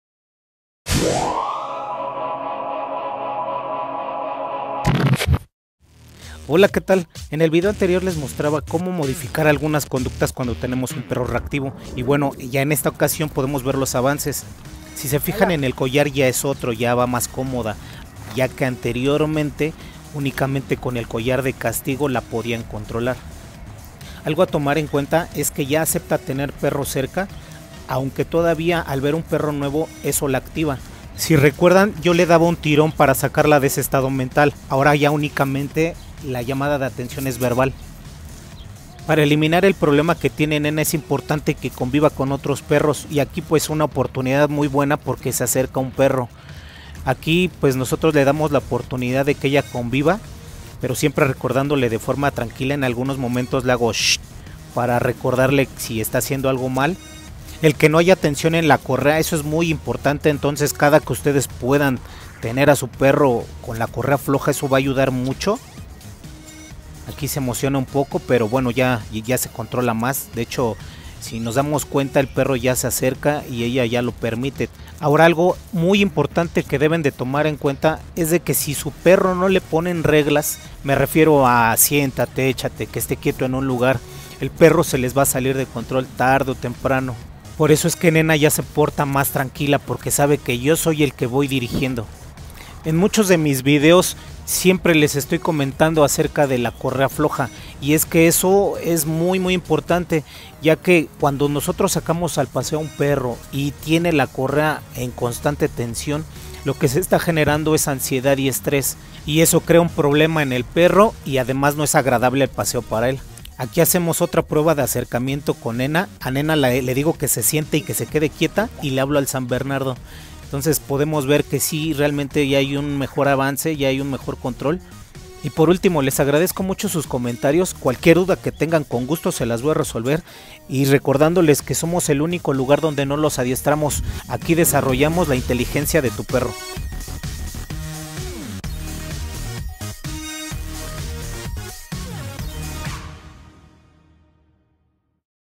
Rehabilitando Perro “PITBULL AGRESIVO”
2a parte de la rehabilitación con algunos Tips para este tipo de problemas